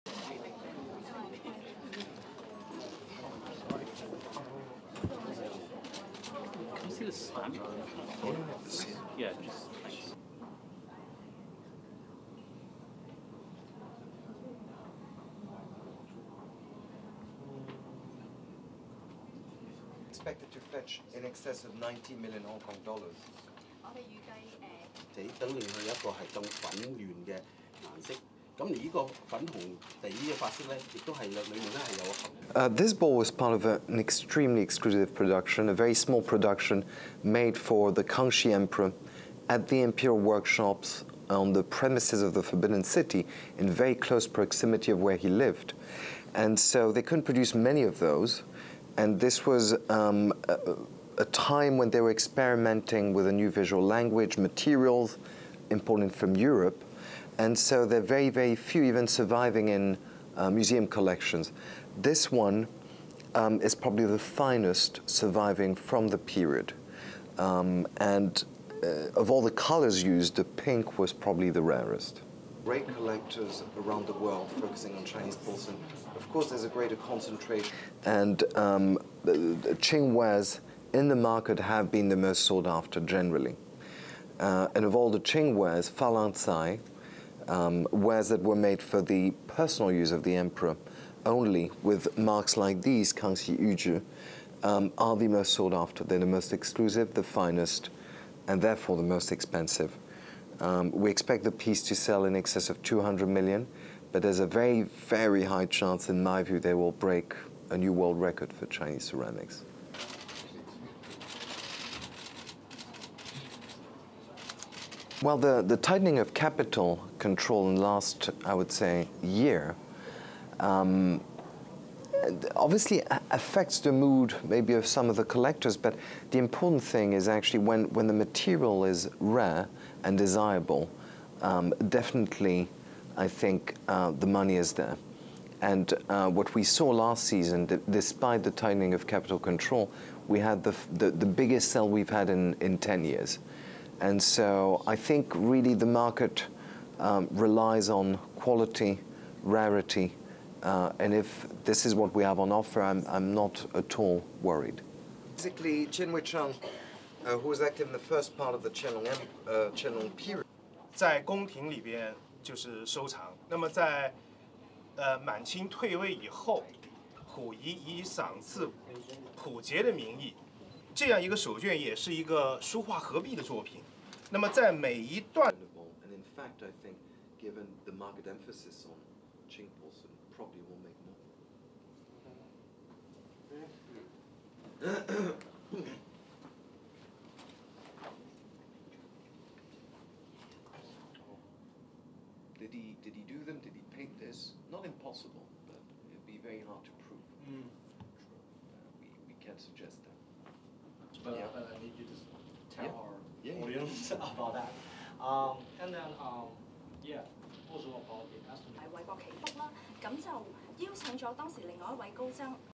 Rare Qing dynasty enamel bowl set to break records at auction
(1 Mar 2018) LEAD IN:
A rare bowl made for the longest-reigning Chinese emperor is expected to be sold for a record-breaking price at auction.
The gold and pink enamel bowl is one of only a few of its kind made for the Kangxi Emperor and has an estimated sales price of more than 25 million US dollars.

STORY-LINE
This delicate enamel bowl is set to become a record breaker.
Sotheby's auction house expects it will become the most expensive Chinese ceramic item ever sold when it goes under the hammer in April.
It's expected to sell for more than 200 million Hong Kong dollars (25.6 million US dollars).
The Qing Dynasty bowl is extremely rare, says Nicolas Chow, Sotheby's International Head of Chinese Works of Art.
"This bowl was part of an extremely exclusive production, a very small production, made for the Kangxi Emperor at the imperial workshops on the premises of the Forbidden City, in very close proximity of where he lived," he says.
"And so they couldn't produce many of those, and this was a time when they were experimenting with a new visual language, materials imported from Europe. And so there are very very few even surviving in museum collections. This one is probably the finest surviving from the period, and of all the colours used, the pink was probably the rarest."
He says it's the only bowl ever recorded with this design.
A similar bowl exists in Taipei with the same rare colours but a different floral design, Sotheby's says.
The Kangxi Emperor, who reigned from 1661 to 1722, was interested in ceramics and art.
But the pink and blue enamel imported from Europe were not commonly used in Chinese ceramics during the Kangxi period.
The stamp under the bowl shows that it was made exclusively for the emperor's own use - that's why it's so expensive.
"And Qing wares, in the market have been the most sought after generally. And of all the Qing wares, Falancai, wares that were made for the personal use of the Emperor only with marks like these, Kangxi Yu Zhi (translated as made by the imperial order of the Kangxi Emperor), are the most sought after, the most exclusive, the finest, and therefore the most expensive" Chow says.
"We expect the piece to sell in excess of 200 million (Hong Kong dollars), but there's very very high chance in my view it will break a new world record for Chinese ceramics."
The auction house said the bowl was created by potters in the Imperial kilns in Jingdezhen and then painted in the Forbidden City in the Imperial Palace workshops.
Sotheby's says the bowl was once owned by the collector Henry M. Knight and has not been up for sale for more than 30 years.
Chow says he is not worried that the tightening of capital controls will affect sales of Chinese art.
"Well the tightening of capital control in the last, I would say, year, obviously affects the mood maybe of some of the collectors, but the important thing is actually when the material is rare and desirable, definitely I think the money is there," he says.
"And what we saw last season, despite the tightening of capital control, we had the biggest sale we've had in 10 years. And so I think really the market relies on quality, rarity, and if this is what we have on offer I am not at all worried."
Other highlights of the sales will be a Chinese ink handscroll, "Ten Auspicious Landscapes of Taishan" by Qian Weicheng, estimated at between 6.41 and 8.97 million US dollars, and two Buddhist Sutra albums made in the Ming Dynasty.
The bowl and other items, along with other Chinese works of art, will be sold in the auction at the Hong Kong Convention and Exhibition Centre on 3 April 2018.